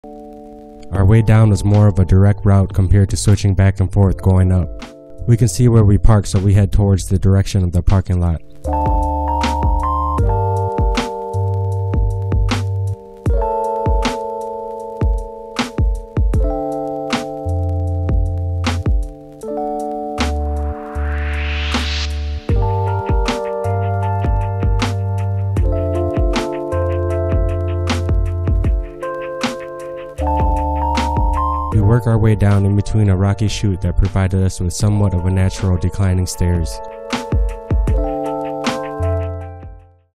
Climbing Calico Basin in Red Rock Canyon, Las Vegas
Calico Basin is half an hour from the Las Vegas Strip. One of my favorites at Red Rock. 360 views at the top. No trail, just scramble up. Always end up coming back to this adventure.